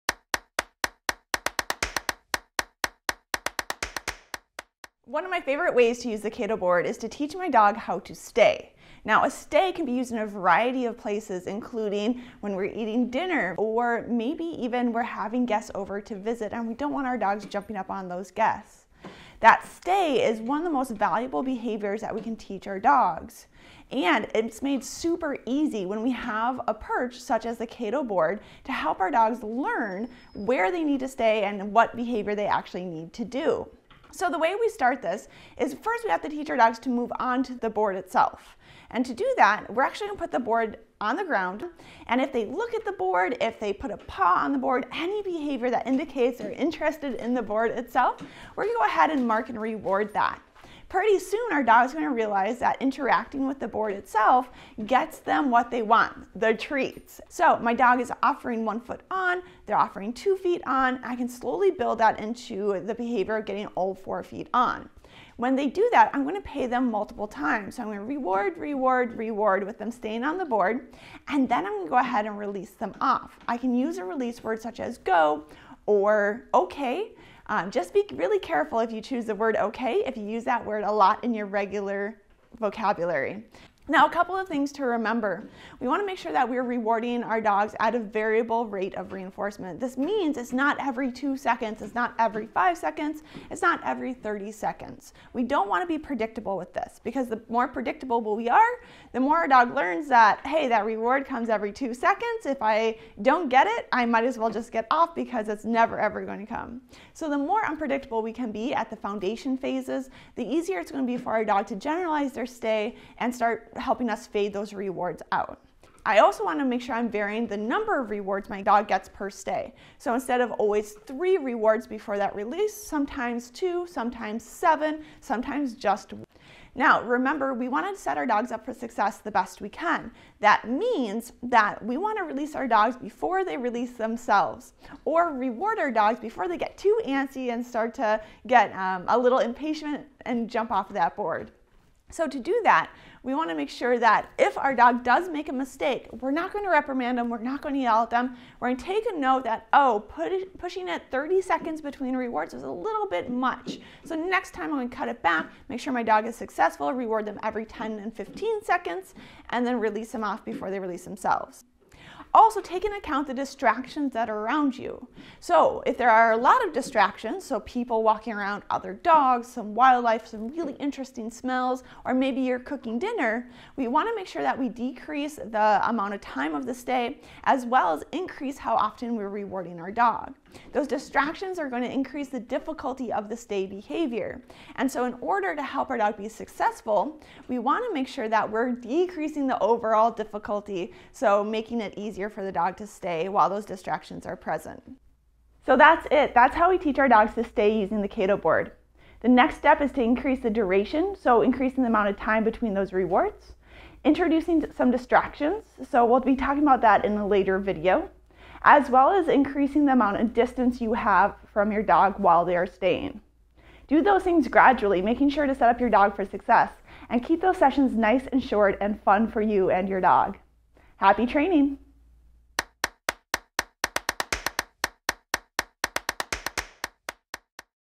Teach your dog to STAY using Cato Boards!
Sara Brueske with ZoomDog training shows how to use Cato Boards to teach "stay". Sara makes it easy to teach this important command using Cato Boards!